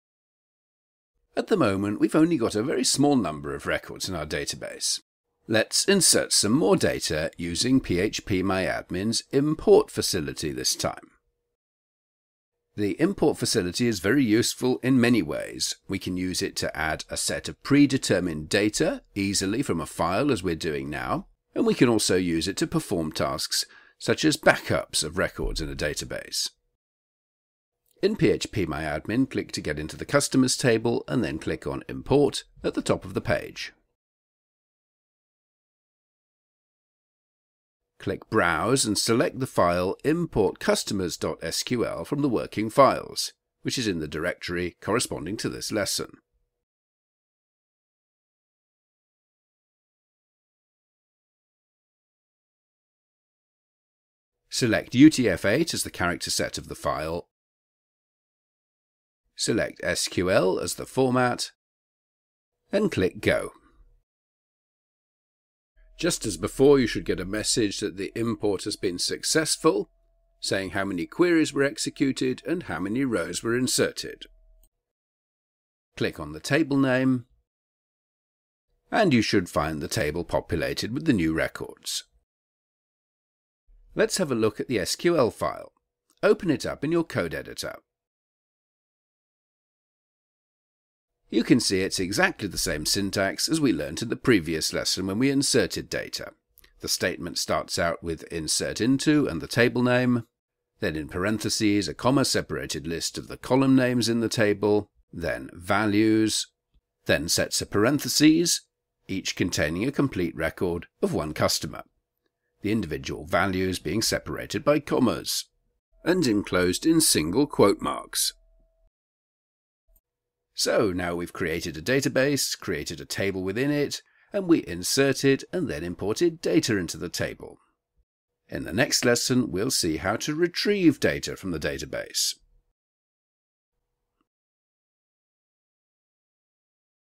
Practical MySQL 01-04 Import data from SQL file
How to import data from an SQL file into a MySQL database.
This is a free lesson from my course Practical MySQL.